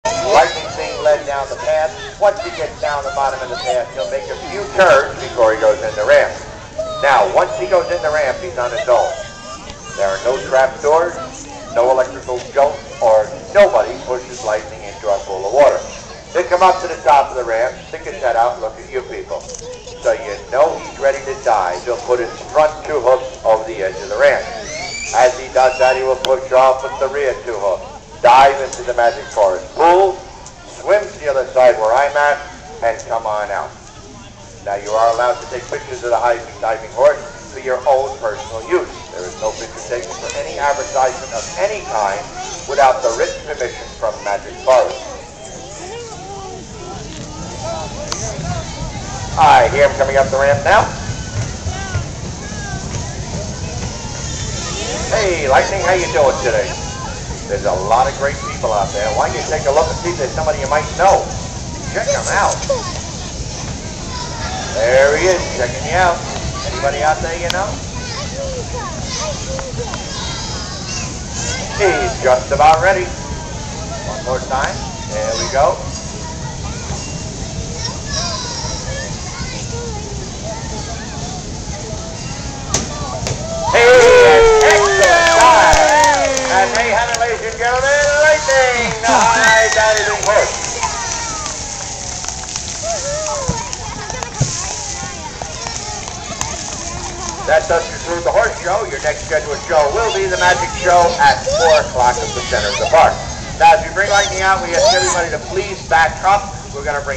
Shocking Diving Horse Stuns Crowd at Magic Forest Lake George NY
This is Lightning the Diving Horse performing at Magic Forest in Lake George, NY. This video is from 2008. This act is no longer performed. Magic Forest is now called Lake George ExpeditionPark.

No rider. No prods. No electrical jolts. No trap doors.